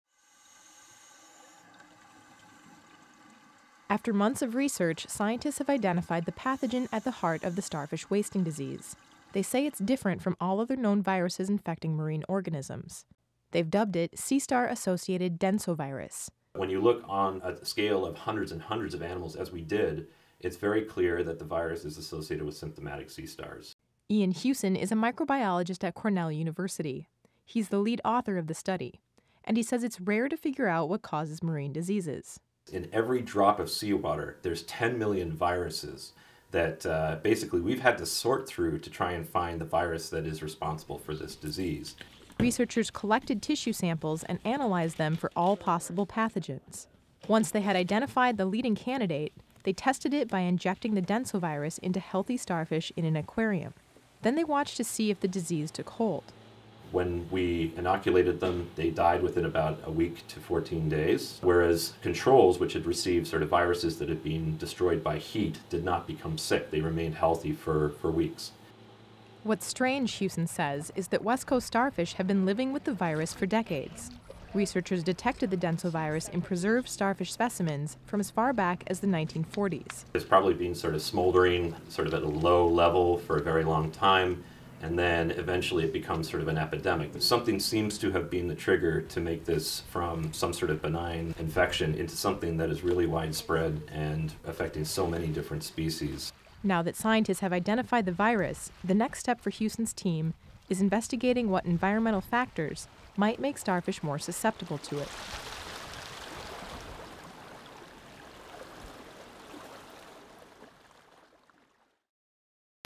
Scientists Solve Mystery Of West Coast Starfish Die-Off
After months of research, scientists have identified the pathogen at the heart of the starfish wasting disease that’s been killing starfish by the millions along the Pacific shores of North America, according to research published Monday.

They said it’s a virus that’s different from all other known viruses infecting marine organisms. They’ve dubbed it “sea star associated densovirus.”